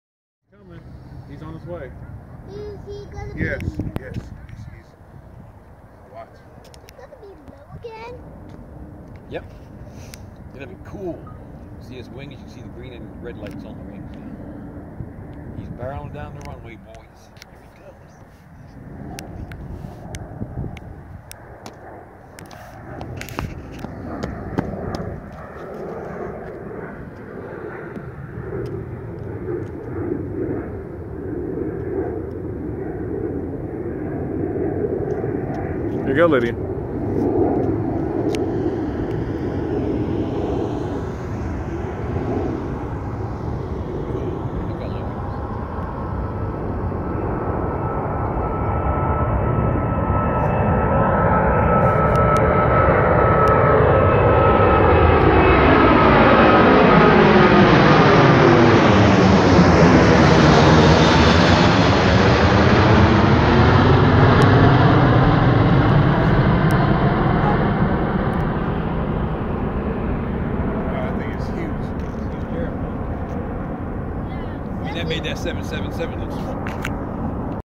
Quantas Airbus A380-8 takeoff
Quantas Airbus A380-8 takeoff from DFW on 12/29/2014